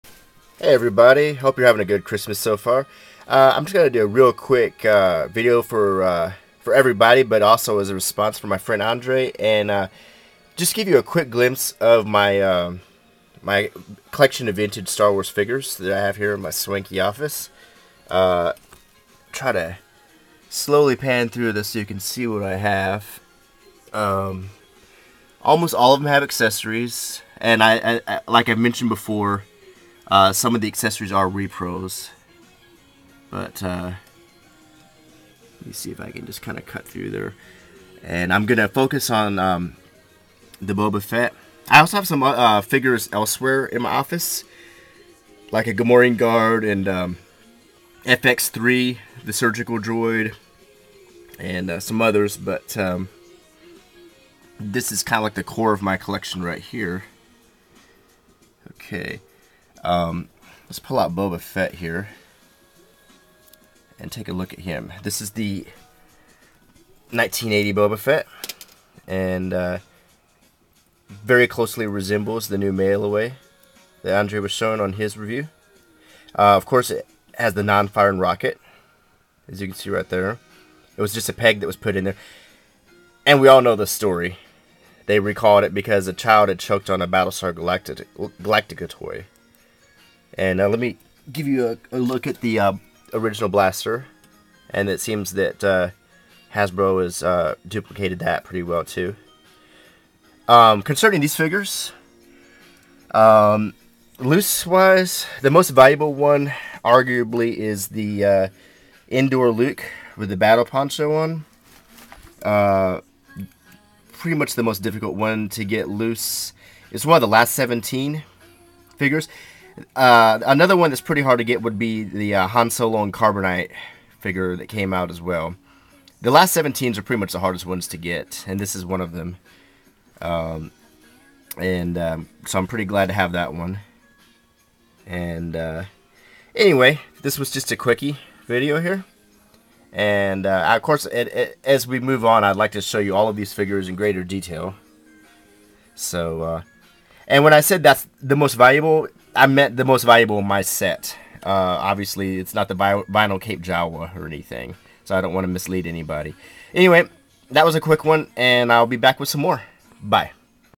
A quick look at my vintage Star Wars collection
Just a quick look at my complete collection of loose figures and a close look at the 1980 Boba Fett.